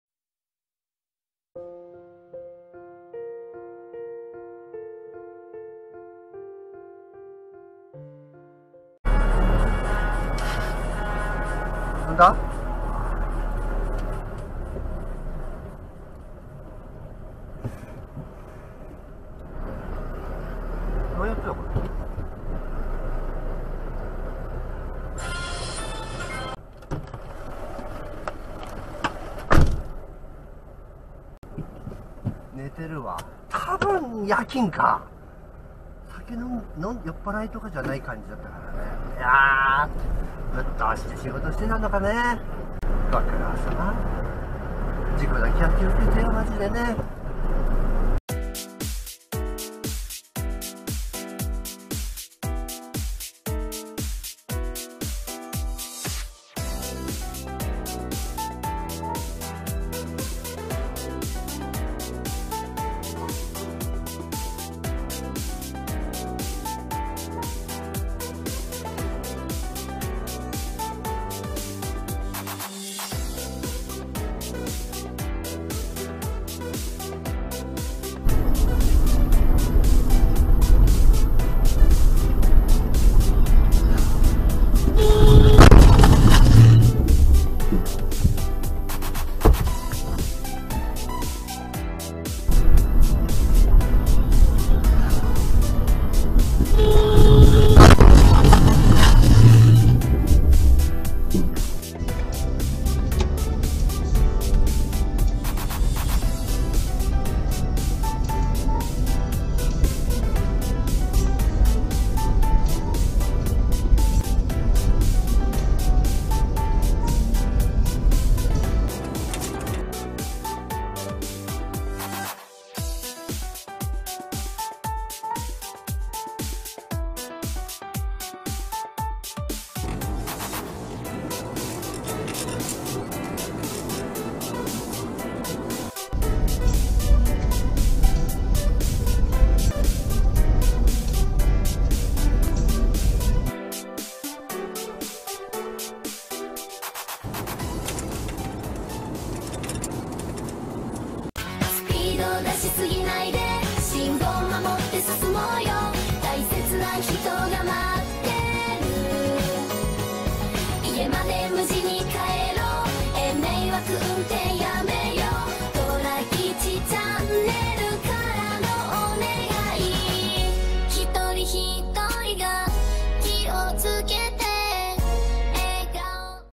迷惑運転者たち No.1927 外国人同士のケンカ・・路上で揉める！・・【危険運転】【ドラレコ】【事故】
１７０度広角カメラにて撮影しているため

amazon.co.jp/dp/B0C6DR8W73
amazon.co.jp/dp/B0BWH4K2RF